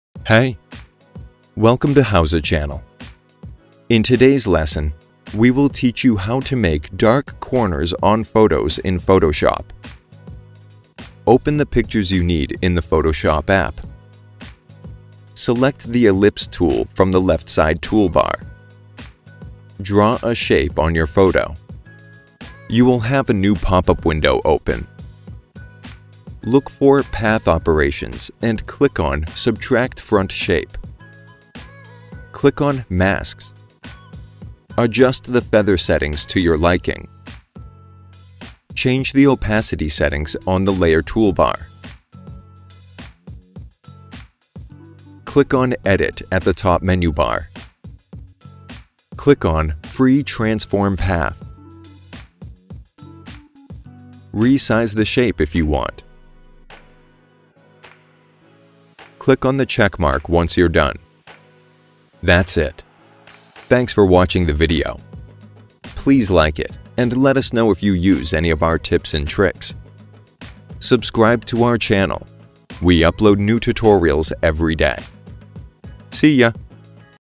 How to Make Dark Corners on Photos in Photoshop
In today's lesson, we will teach you how to make dark corners on photos in Photoshop.
Open the pictures you need in the Photoshop app. Select ‘Ellipse’ tool from the left side tool bar. Draw a shape on your photo.
You will have a new pop-up window open. Look for Path operations and click on ‘Subtract front shape’. Click on ‘Masks’. Adjust Feather settings to your liking.
Change Opacity settings on the Layer tool bar.
Click on Edit at the top menu bar. Click on ‘Free transform path’. Resize the shape if you want. Click on the checkmark once you’re done.